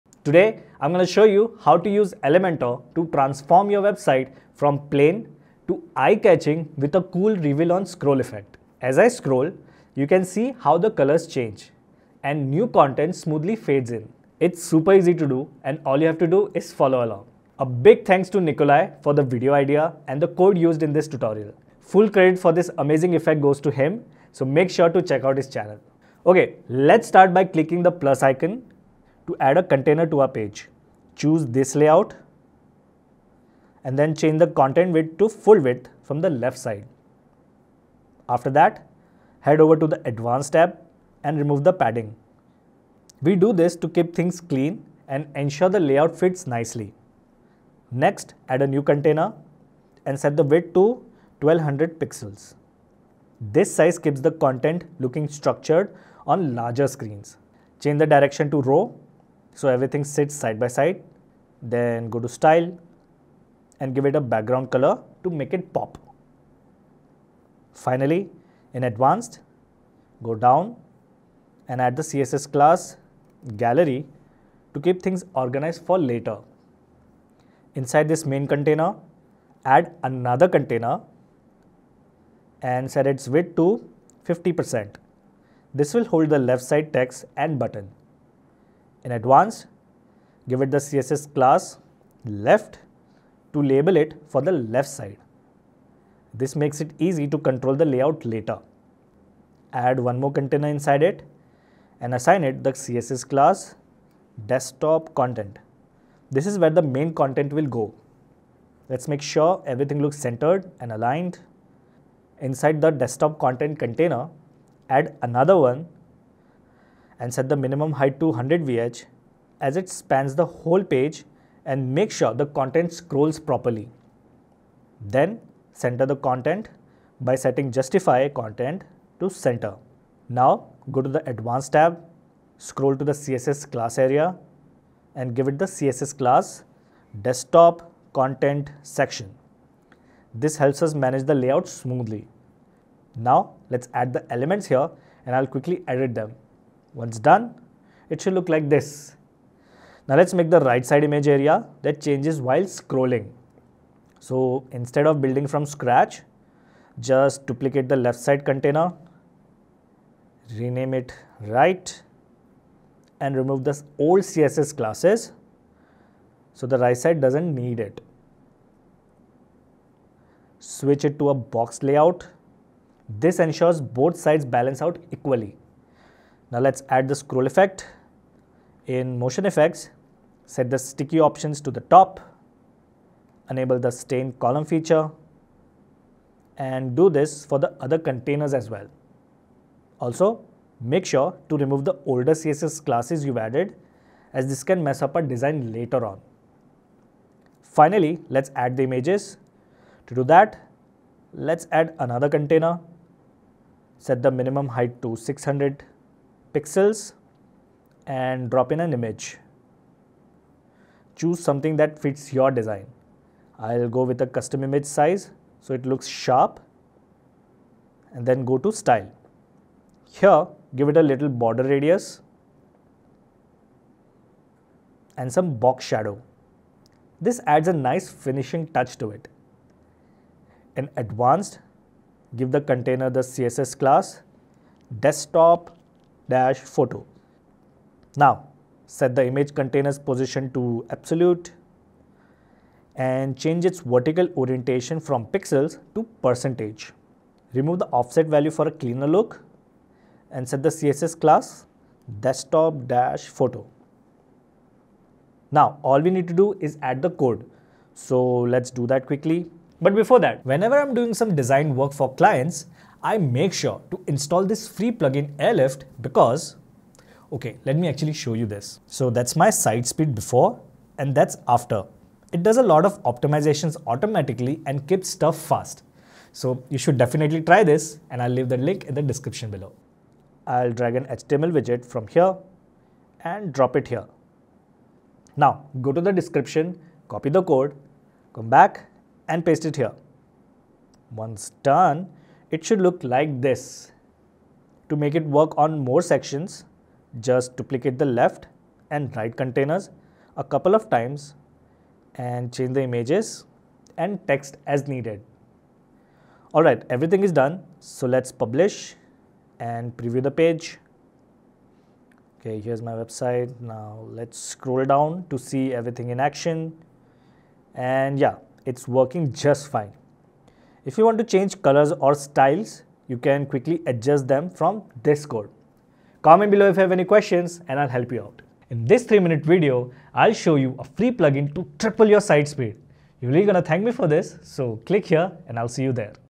Change Image & Text on Scroll - Elementor GSAP Scrolltrigger
⭐ Giveaway: Our 100% FREE plugin makes any site 3X faster In this video, I’ll show you how to use Elementor to create a sleek reveal-on-scroll effect to make your website stand out. We’ll walk through adding containers, adjusting layout settings, and using custom CSS classes to ensure everything looks great on all screen sizes. I’ll guide you on how to set up a left-side content section and a right-side image area that changes as you scroll. Plus, I’ll share the custom code provided by Nicolai Palmkvist, who inspired this tutorial. By the end, you’ll know how to add images, style them with border-radius and shadows, and use motion effects to create a dynamic user experience.

00:00 How to reveal content on scroll?
00:28 Create foundation (Dont miss or you'll get errors)
04:17 Add reveal content on scroll effect